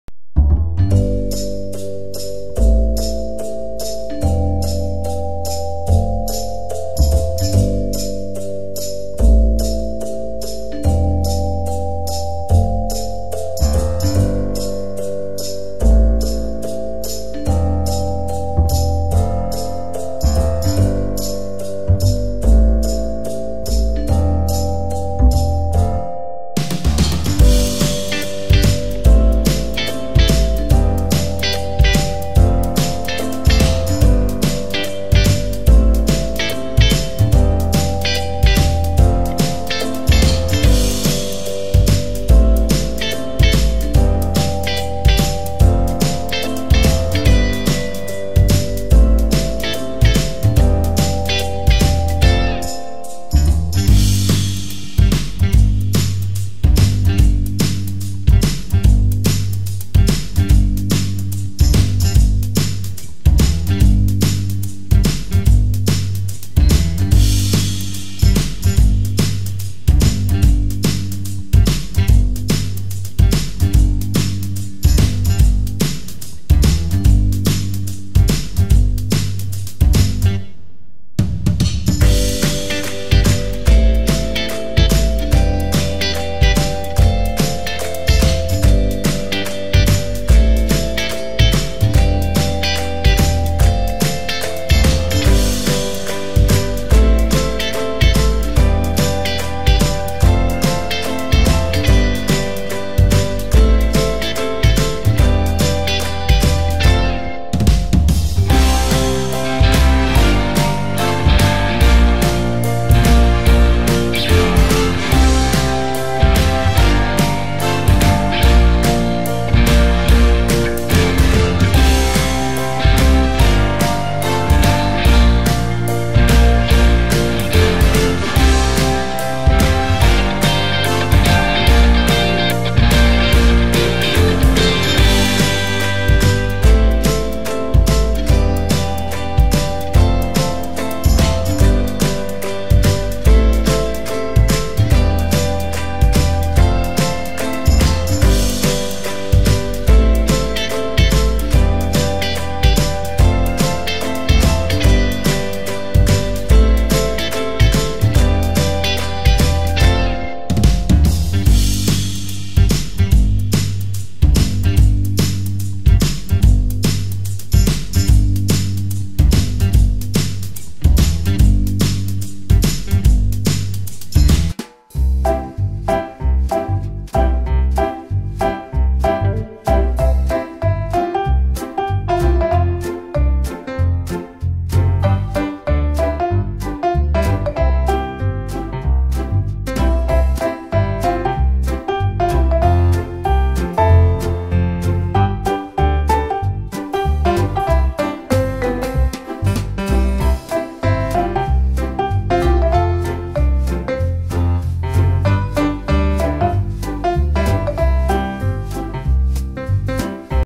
How to Draw shirt, hat and sandals drawing, Painting and Coloring for Kids & Toddlers
How to Draw shirt, hat and sandals drawing, Painting and Coloring for Kids & Toddlers | Draw, Paint and Learn